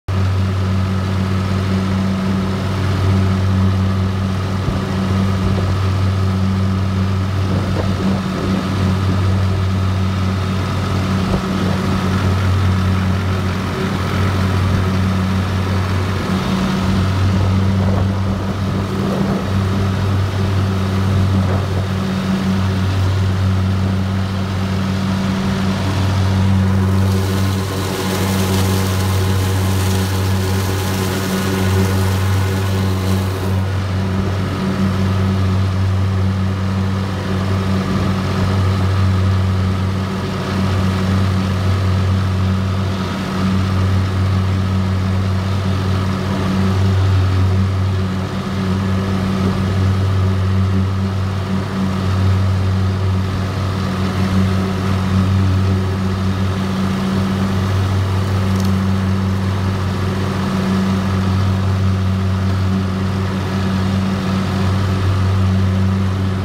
2605H Massey Ferguson with Woods finish mower
Mowing behind the dam, and yes, the Massey Ferguson is quieter than the Deere.